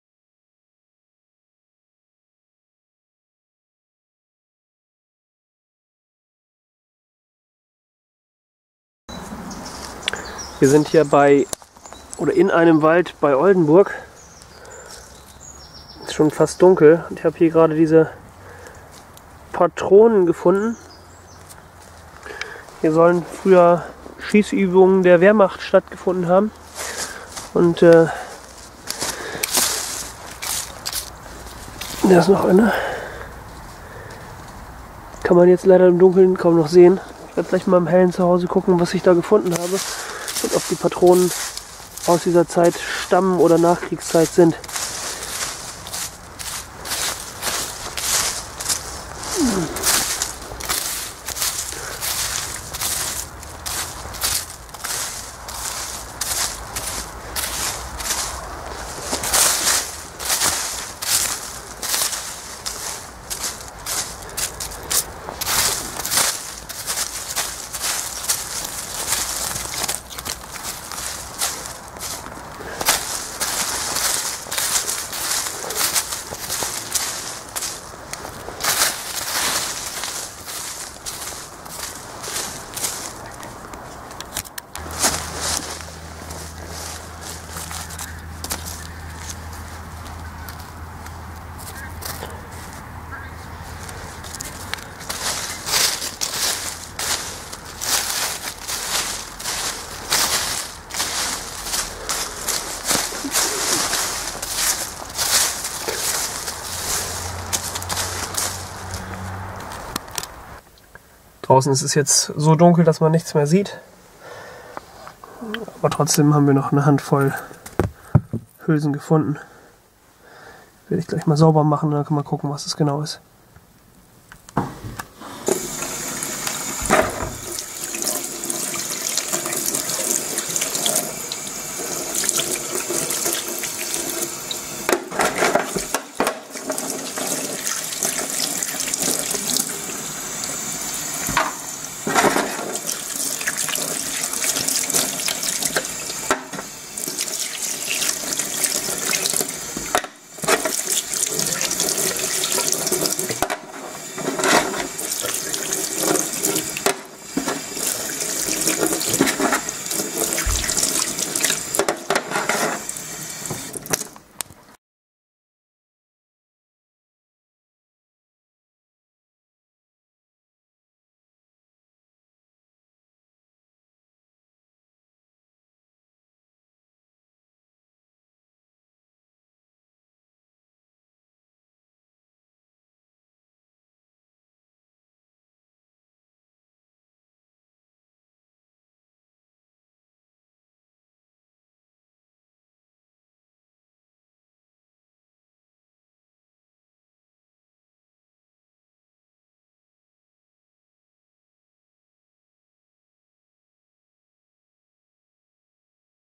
WW2 Relikte Teil 66 : Einzelne Hülsen WK2 nähe Oldenburg (Wehrmacht)
Einzelne Hülsen von einer Stelle in der Nähe von Oldenburg. Hier soll die Wehrmacht während des WK2 eine Übung und am Ende des Krieges die Erschießung von Desateuren durchgeführt haben. An den Bäumen entlang der Straße waren ebenfalls Fahnenflüchtige aufgehängt.